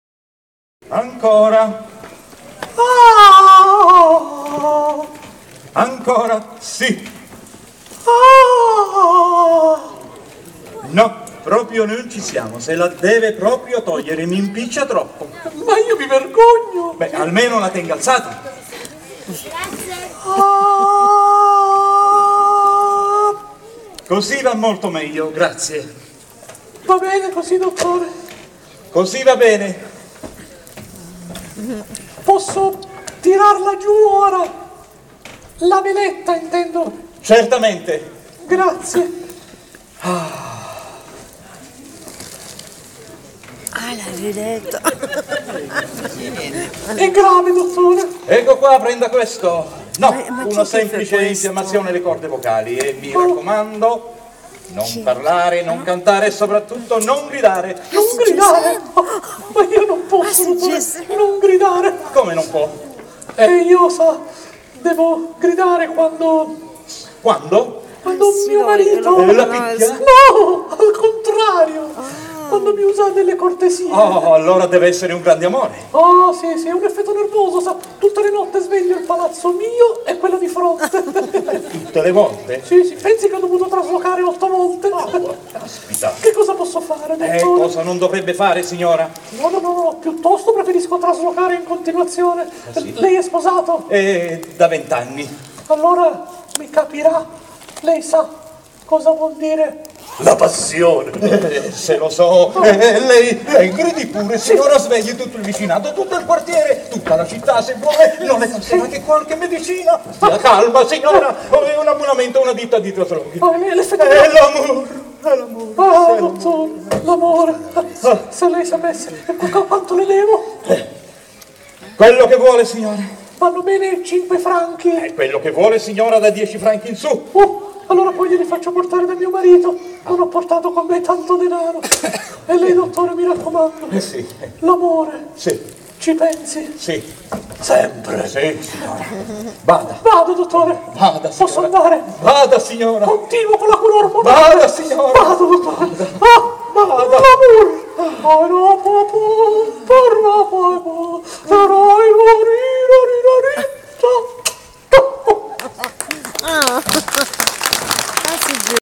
Le pillole di Ercole (M. Hennequin e P. Bilhaud)
La prima scena sul palco di Furnari il 12 Settembre 2015
Teatro Stabile Milazzo con Giuseppe Pollicina e Santi Puliafito